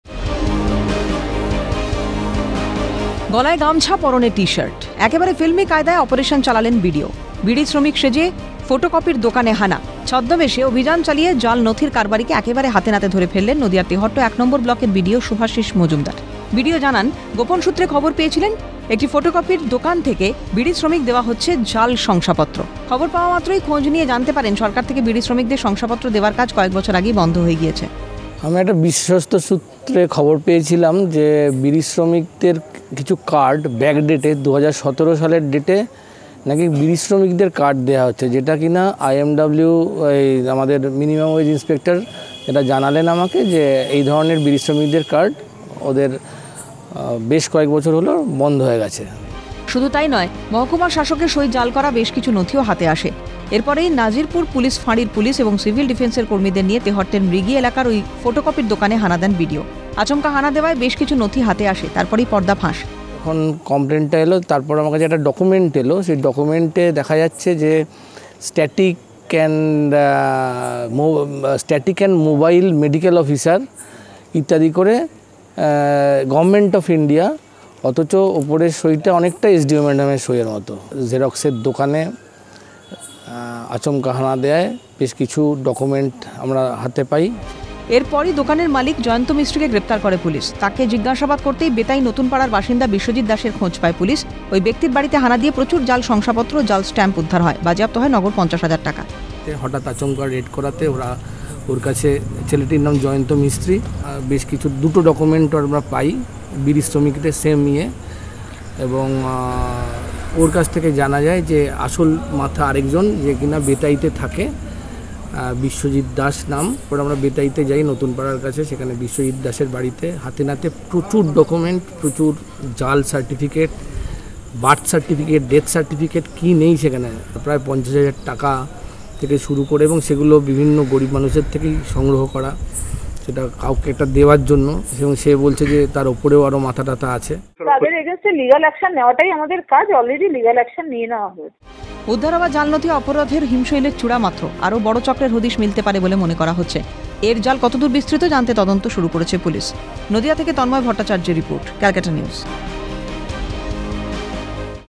বিড়িশ্রমিক সেজে জেরক্সের দোকানে হানা বিডিওর | ক্যালকাটা নিউজ
বিড়িশ্রমিক সেজে জেরক্সের দোকানে হানা বিডিওর

বিড়িশ্রমিক সেজে জেরক্সের দোকানে হানা।  ছদ্মবেশে অভিযান চালিয়ে জাল নথির কারবারিকে একেবারে হাতেনাতে ধরে ফেলেন নদিয়ার তেহট্ট ১ নম্বর ব্লকের বিডিও শুভাশিস মজুমদার।